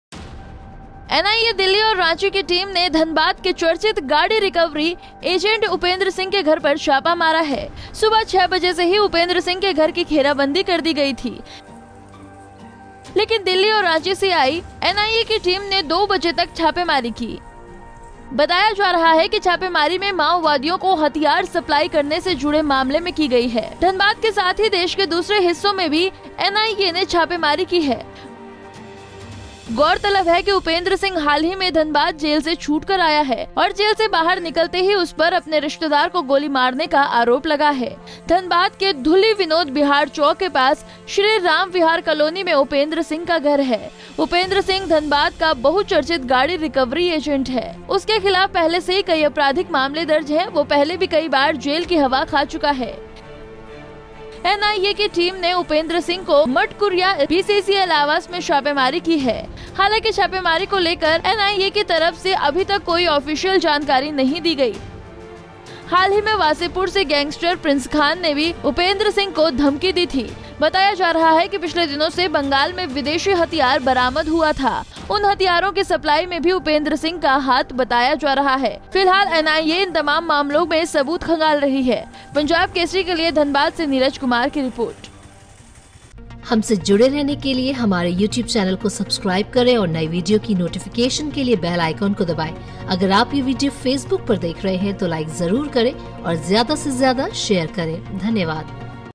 धनबाद के कुख्यात गाड़ी रिकवरी एजेंट उपेंद्र सिंह के घर NIA की टीम ने मारा छापा
nianedhanbadmemarachapa NIA ने रिकवरी एजेंट के घर मारा छापा
एनआइए दिल्ली और रांची की टीम ने धनबाद के चर्चित गाड़ी रिकवरी एजेंट उपेंद्र सिंह के घर पर छापा मारा है. सुबह छह बजे से ही उपेंद्र सिंह के घर की घेराबंदी कर ली गई थी. लेकिन दिल्ली और रांची से आई एनआईए की टीम ने दो बजे तक छापेमारी की. बताया जा रहा है कि ये छापेमारी माओवादियों को हथियार सप्लाई करने से जुड़े मामले में की गई है. धनबाद के साथ ही देश के दूसरे हिस्सों में भी एनआईए ने छापेमारी की है. गौरतलब है कि उपेंद्र सिंह हाल ही में धनबाद जेल से छूटकर आया था. और जेल से बाहर निकलते ही उस पर अपने रिश्तेदार को गोली मारने का आरोप लगा है.